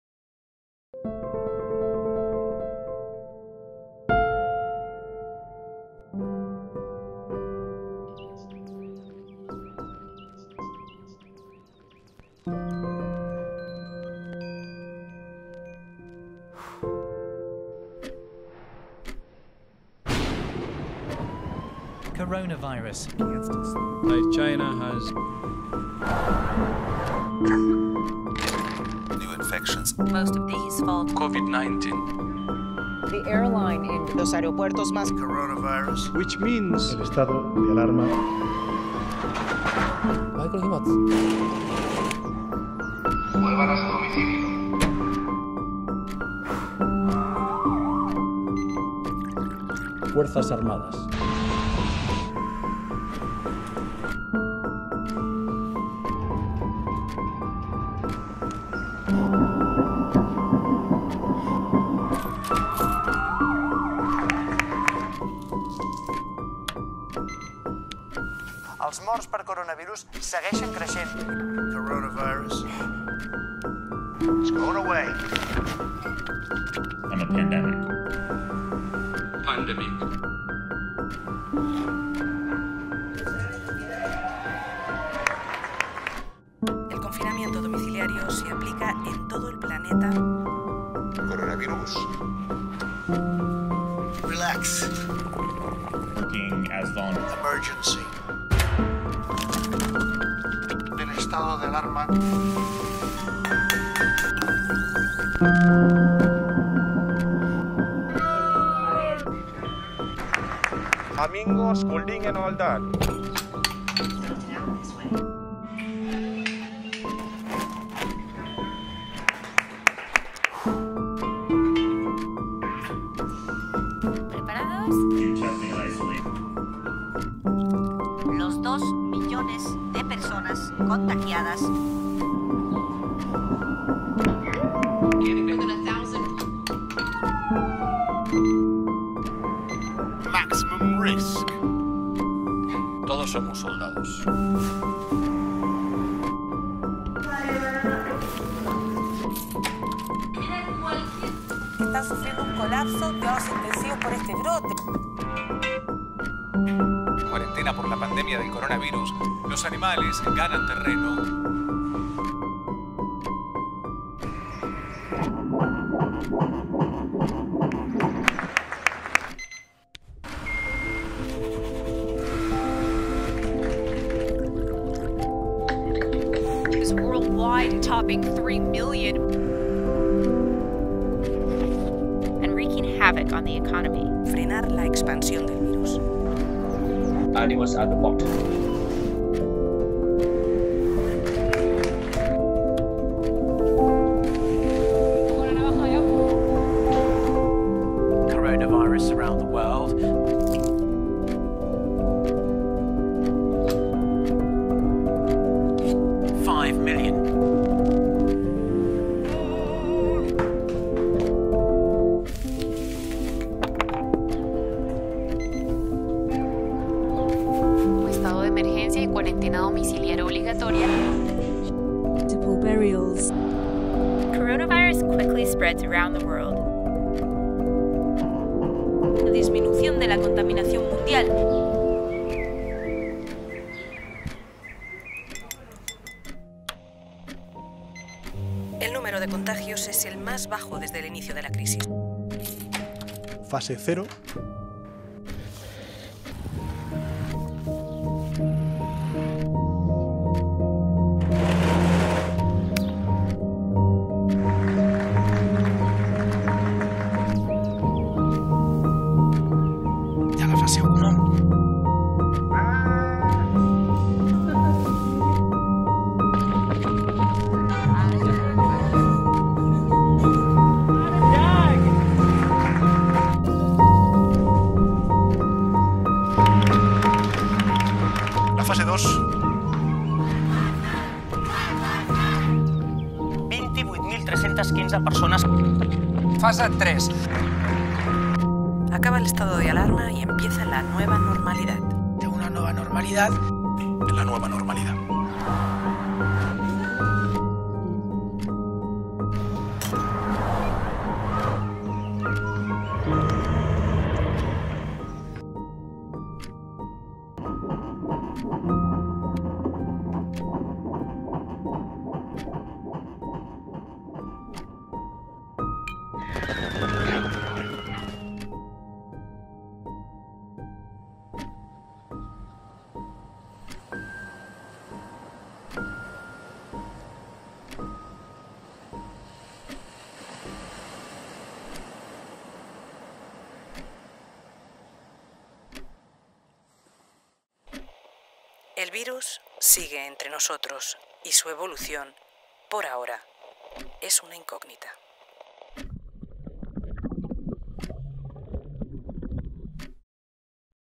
coronavirus # covid19 covidnation coronadays lockdown pandemic. Ray of Light---a lockdown story
This is the story of our confinement. In the middle of the Coronavirus pandemic, my wife and I await the arrival of our first child. And in the middle of the darkness we discover ... a ray of light. Recorded during the 99 days of arlarma state in Sant Just Desvern (March 14 to June 21, Sant Just Desvern, Barcelona). From: Roger Córdoba Schwaneberg, Mary Casiafonica & Ray Music: Lluís Vilaseca Flores Logo Design: Ruben Cordoba Schwaneberg coronavirus # covid19 covidnation coronadays lockdown pandemic fujifilm # fujixt4